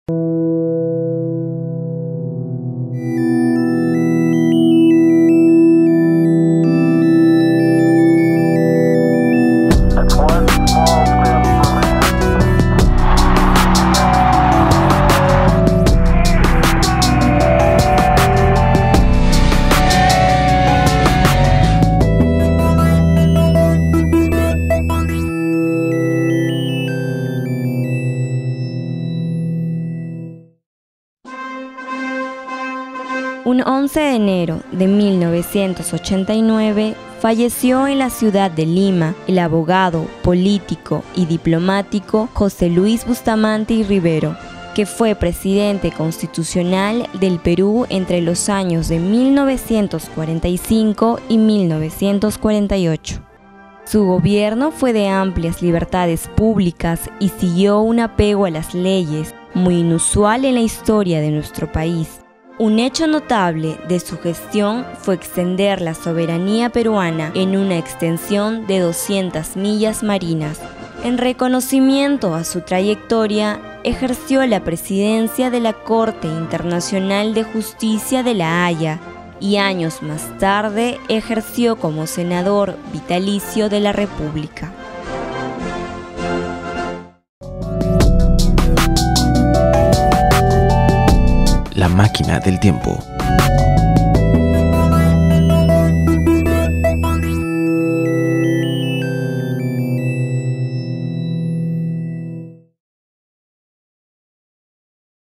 MÁQUINA DEL TIEMPO (11/01/14): FALLECIMIENTO DE LUIS BUSTAMANTE Y RIVERO
Un 11 de enero de 1989 falleció en la ciudad de Lima, el abogado, político, diplomático y escritor nacional, José Luis Bustamante y Rivero; que fue Presidente constitucional del Perú entre los años de (1945 y 1948).